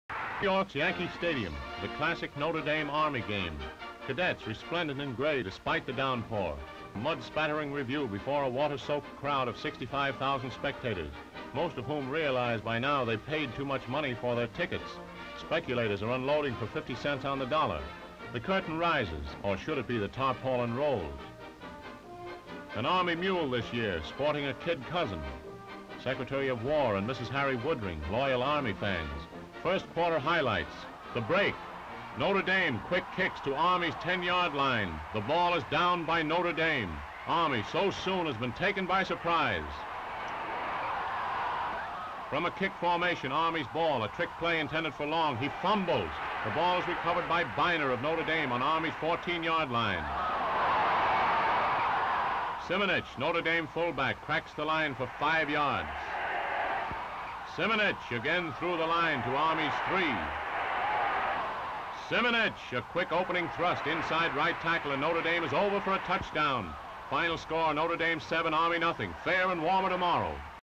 Notre Dame vs. Army 1938 at Yankee Stadium
Classic game clips of the 1938 game won 7 -0 by Notre Dame